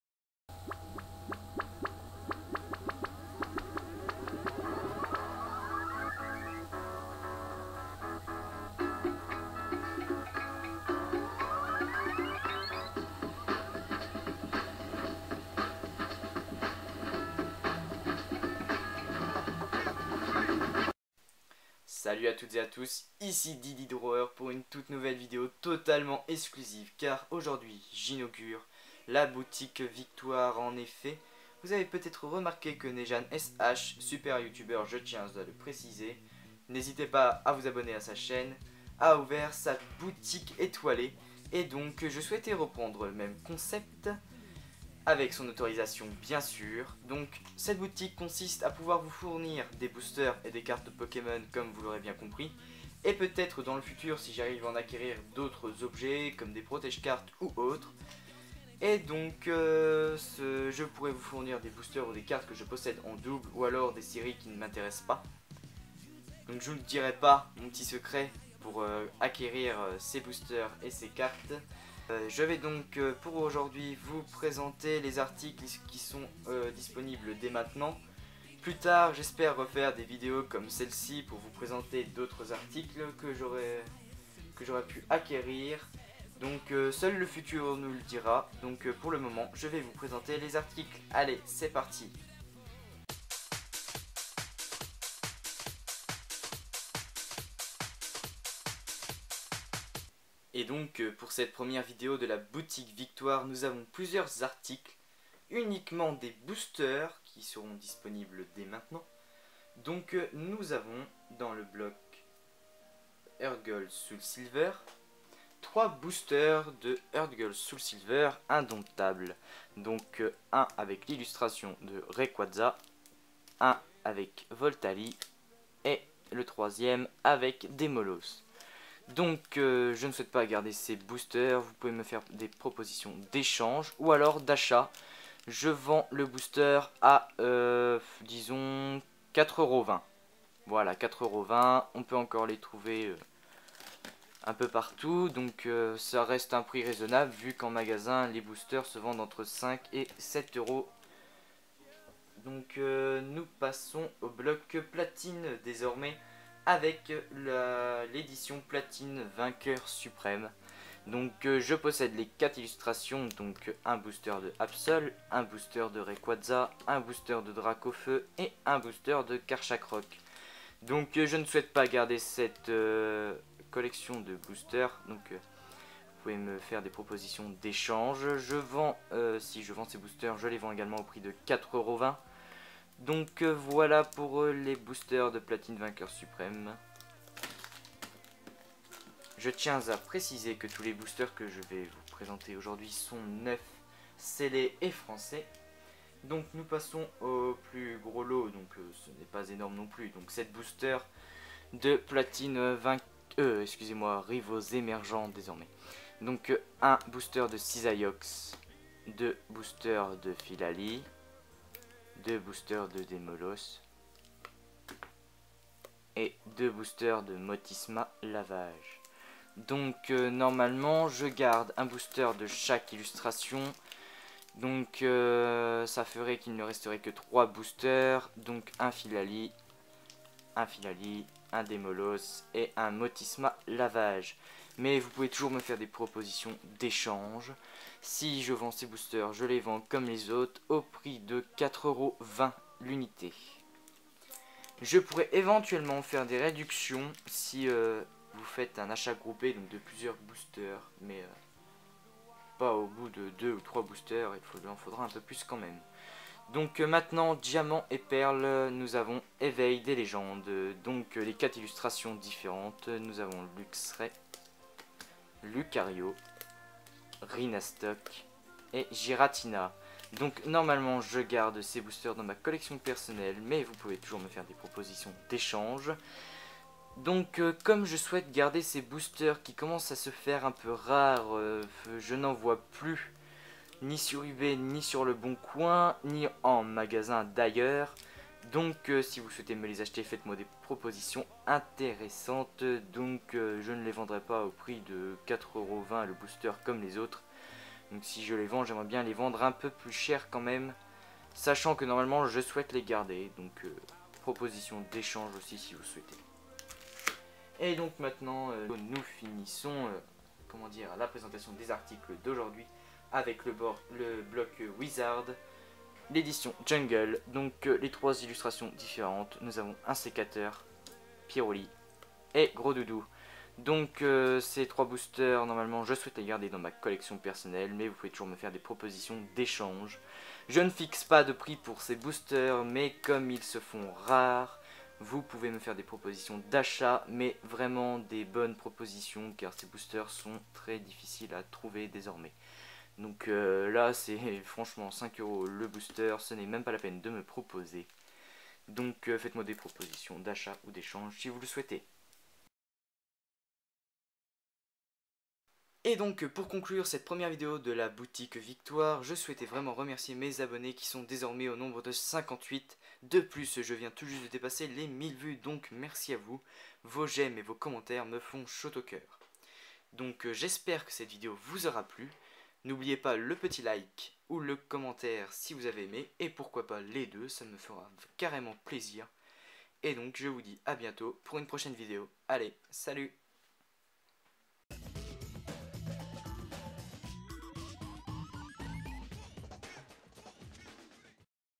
[Boutique] ~ Ouverture de la boutique Victoire ~ Vente & Echange de boosters
Aujourd'hui vidéo un peu spécial car elle traite de l'ouverture de la " Boutique VICTOIRE ". Vous pourrez grâce à cette boutique virtuelle acquérir des objets Pokémon tels que des booster, des cartes et des accessoires.

Pour la première vidéo je vous propose des boosters neufs, scellés et Français de plusieurs séries :

- HGSS - Indomptable = 3 boosters ► Prix unitaire : 4.20€ ou échange
- Platine - Vainqueurs Suprêmes = 4 boosters ► Prix unitaire : 4.20€ ou échange
- Platine - Rivaux Emergents = 7 boosters ► Prix unitaire : 4.20€ ou échange
- D-P - Eveil des Légendes = 4 boosters ► Prix à proposer ou échange
- Wizards - Jungle = 3 boosters ► Prix à proposer ou échange

MES RECHERCHES ( Français / Neuf ou en excellent état ) :

- Tout types de cartes du " set de base " à " Noir & Blanc : Destinées Futures ", exceptées le bloc HGSS.
- Boosters et displays de toutes collections
- Protèges cartes et toploaders

N'oubliez pas le petit LIKE et/ou un commentaire si le coeur vous en dit !

Dydi Drawer ^^